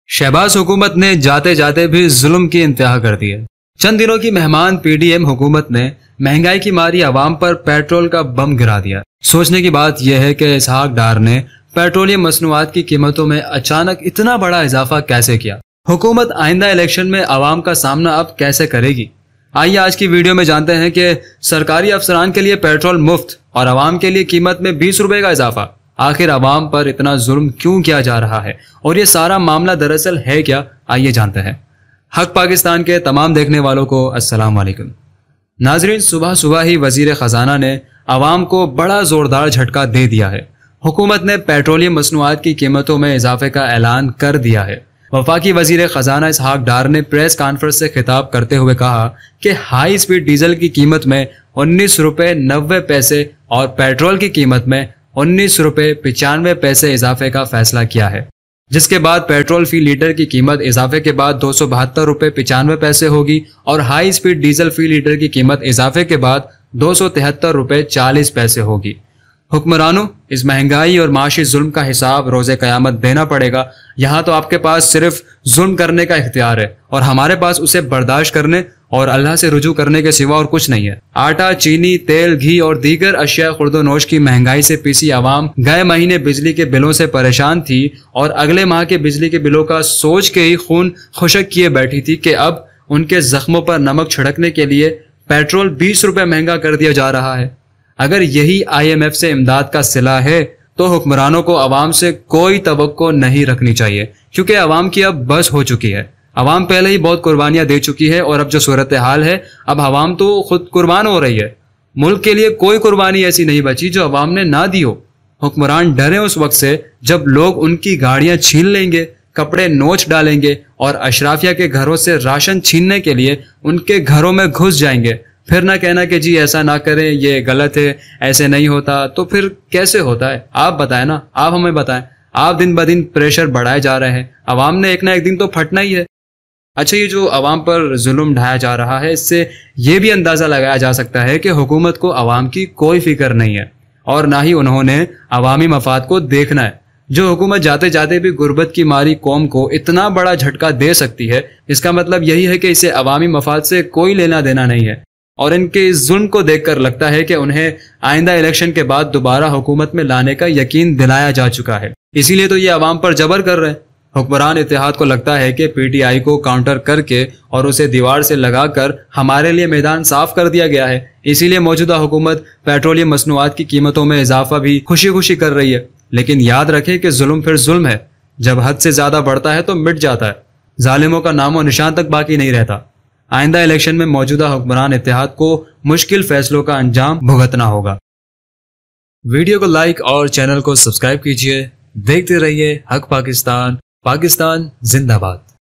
PDM Hukumat Ne Awam Ki Cheekhien Nikal di Petrol Ki Keemat Achanak Bare Izafe Ki Asal Waja Kia?
Ishaq Dar Big Decision | Petroleum Products Latest Updates | Petrol Price Updates | Breaking News

Petrol Prices Increased in Pakistan?

Petroleum dealers association threatens nationwide petrol pump closure

Dealers defer nationwide strike of petrol pumps

Petroleum strike